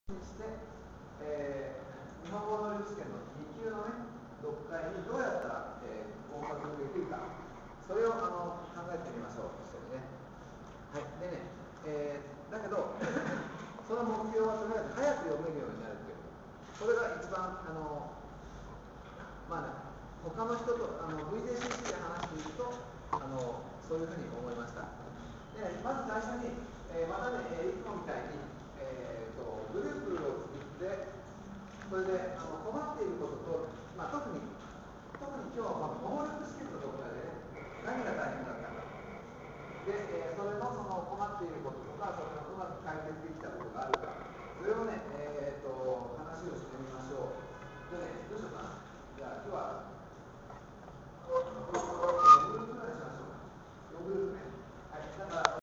ダナン集中日本語教育セミナー 五日目「読解」01
VJCCと国際交流基金の共催のダナン集中日本語教育セミナー五日目の「読解」の授業です。2009年12月25日、ダナン外国語大学で。